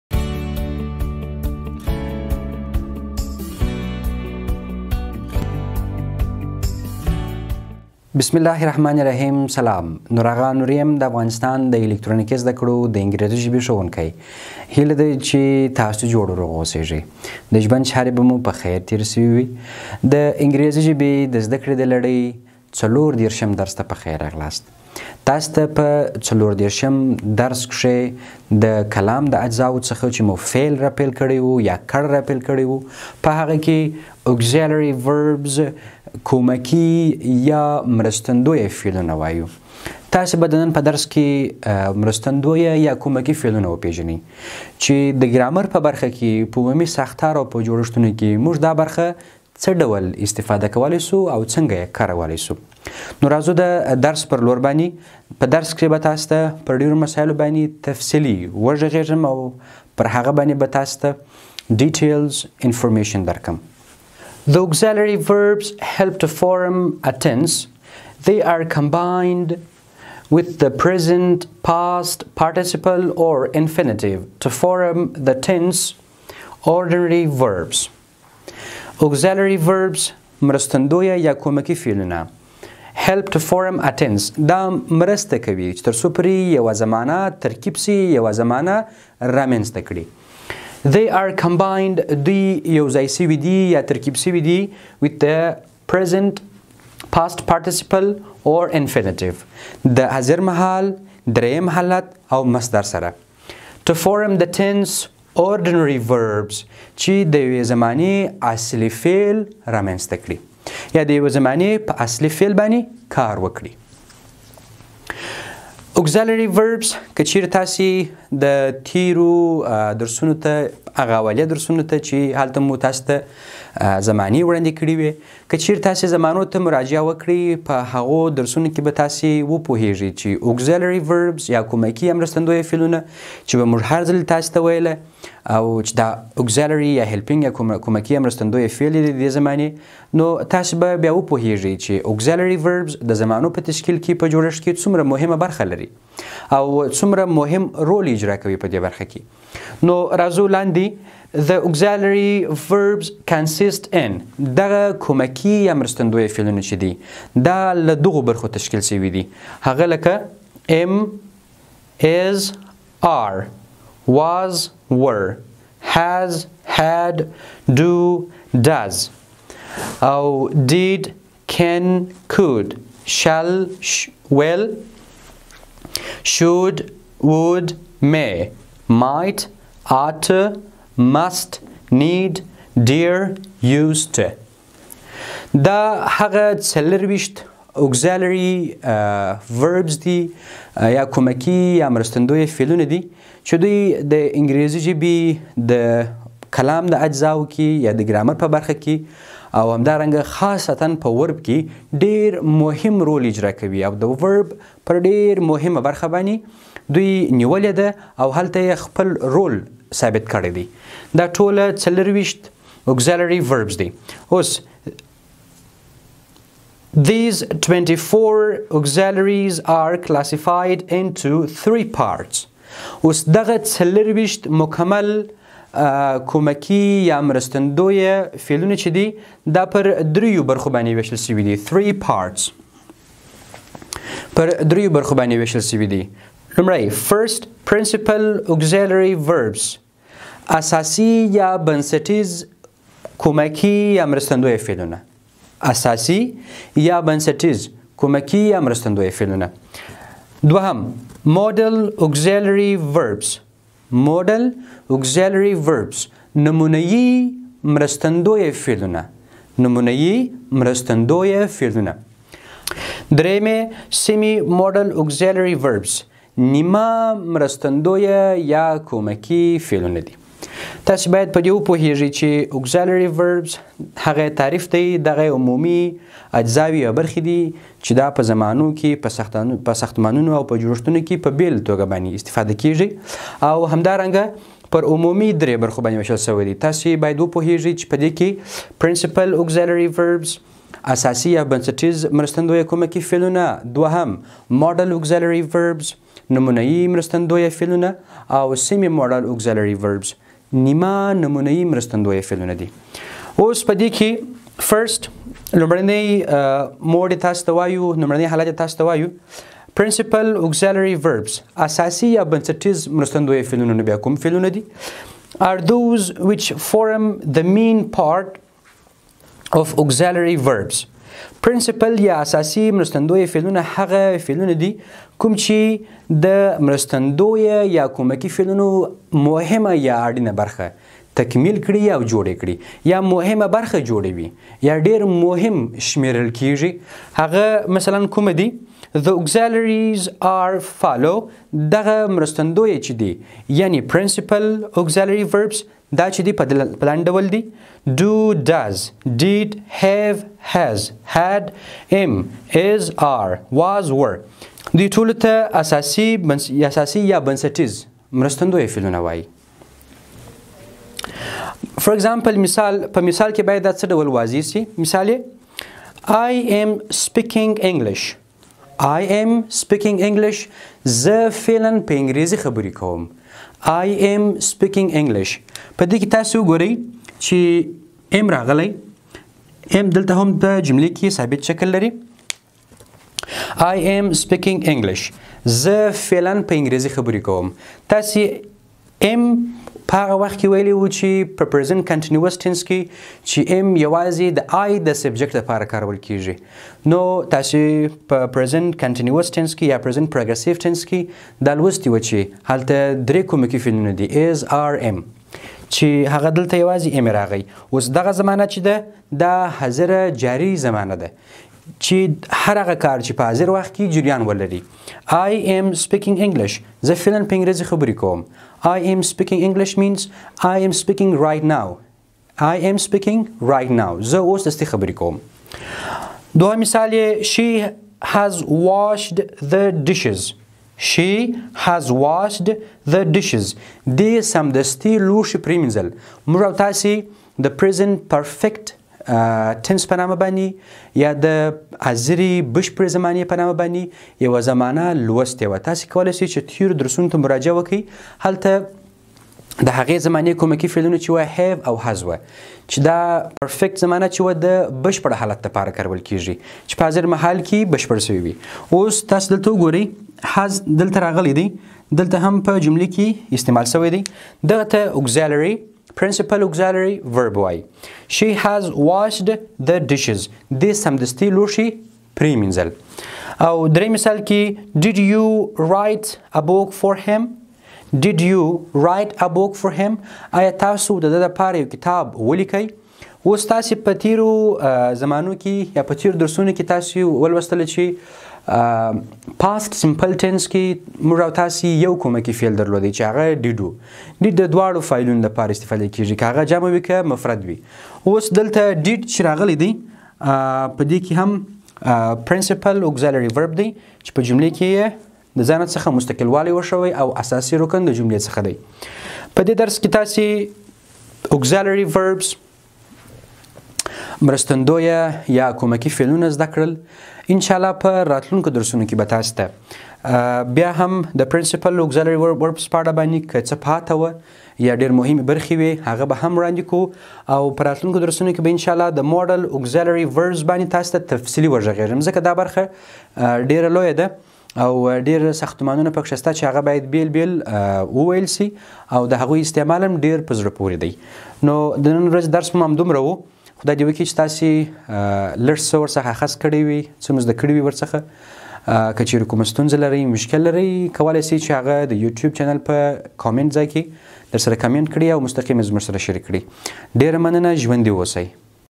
34. Auxiliary Vebs څلورم دېرشم درس: کومکي (مرستندویه) فعلونه
په دې درس کي به تاسي د کلام په اجزاوو کي د فعل یا Verb کي د مرستندویه فعلونو په اړه مفصلاً درس زده کړئ.